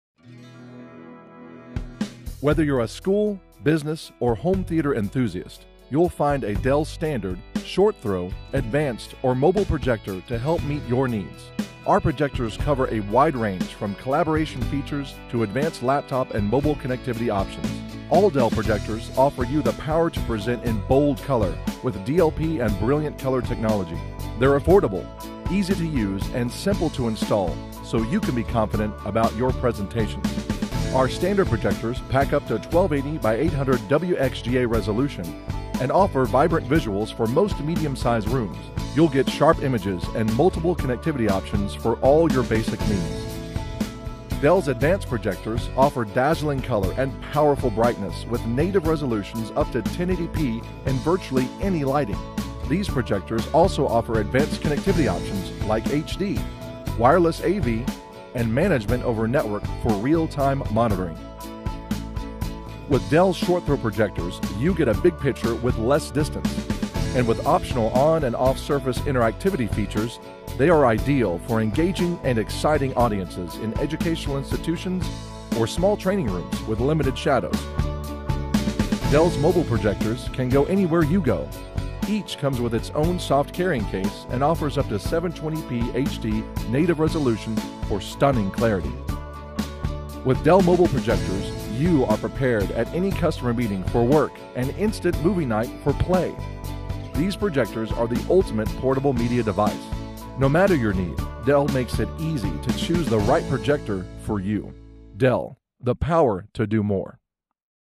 Dell Projectors - Standard, Advanced, Mobile, and Short Throw
Whether you’re presenting your ideas in a boardroom or a classroom, the Dell™ 1210S projector displays graphics and text in bright, high-contrast detail that helps you get your points across.